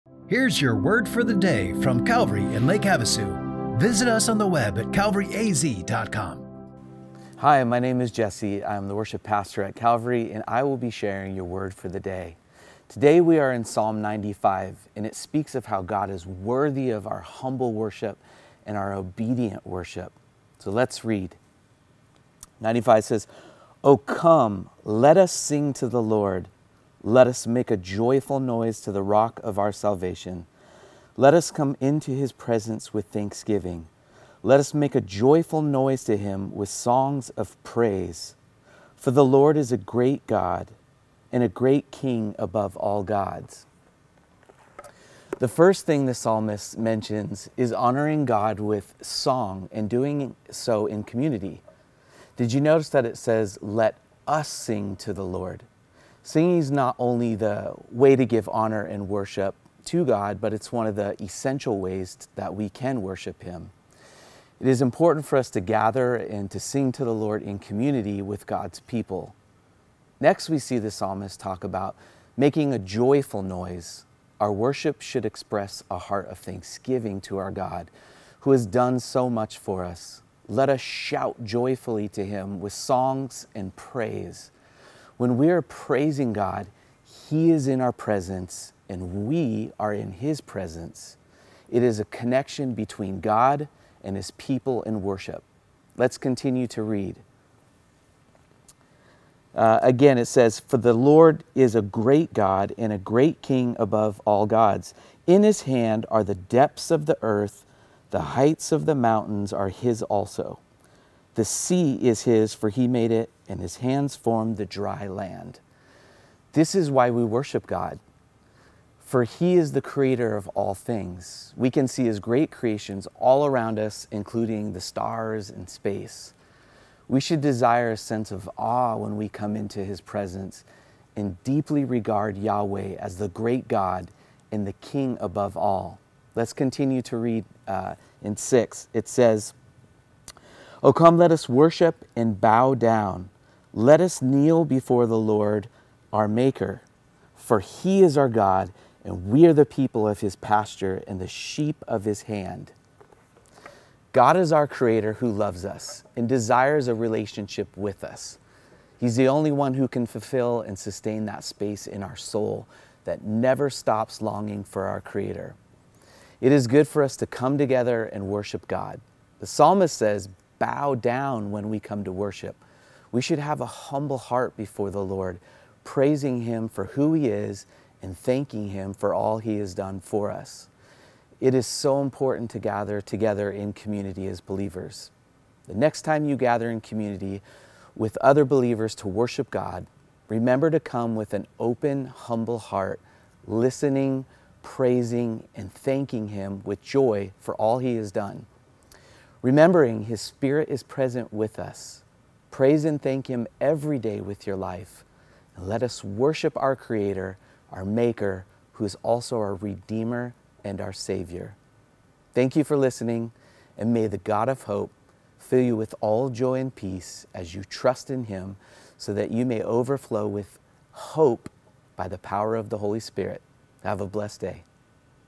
Your Word for the Day - Episode 824
Today Jesse Pruett shares how singing is one of the important ways we honor God with worship.